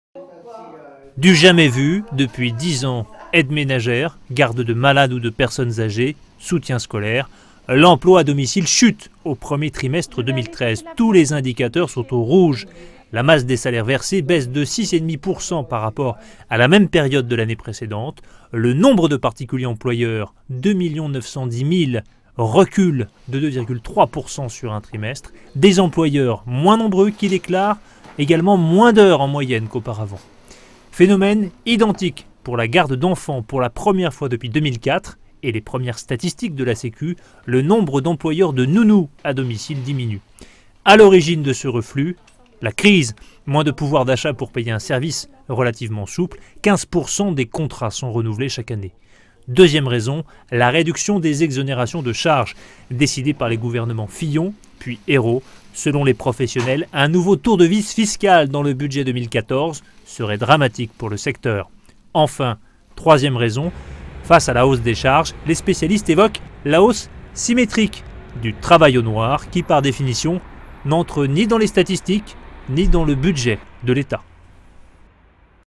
Emplois à domicile : après la chute, le plongeon ?
L’emploi à domicile recule au 1er trimestre, dans des proportions jamais vues depuis 10 ans. Les spécialistes redoutent un nouveau tour de vis fiscal dans le budget 2014, qui pénaliserait le secteur.